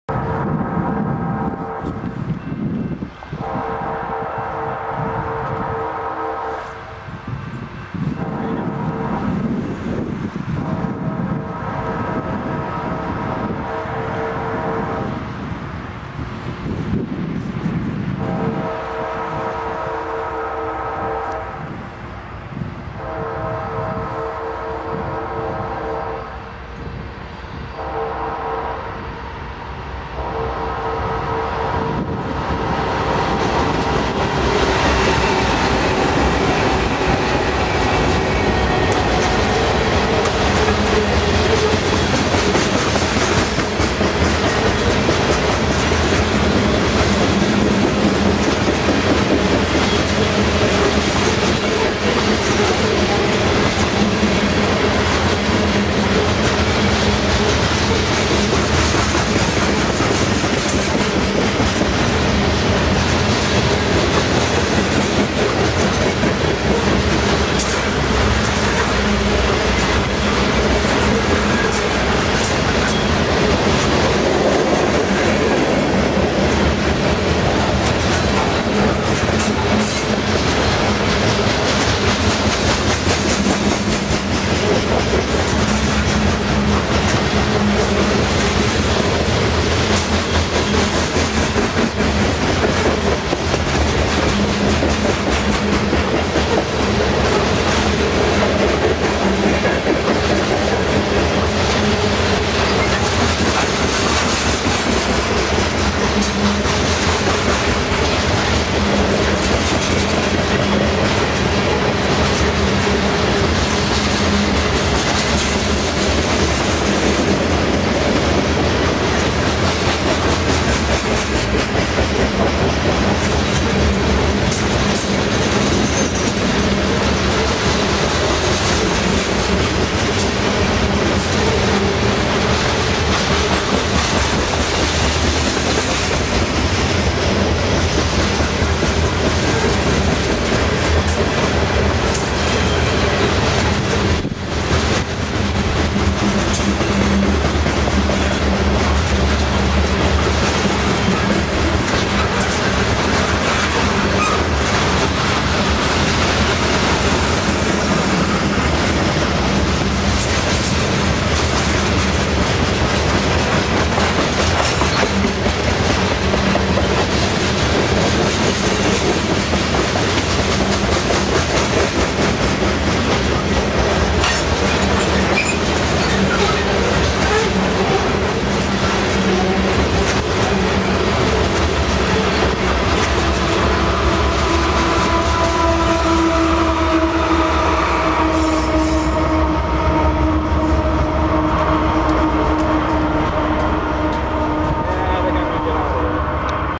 BNSF 725 Leads A Grainer @ Old Town Tacoma, WA w Canon HF11
BNSF 725 and 1118 lead a northbound grainer at Old Town Tacoma, WA on BNSF's Seattle Sub. DPU power is 5710. An 1 March 2014 video.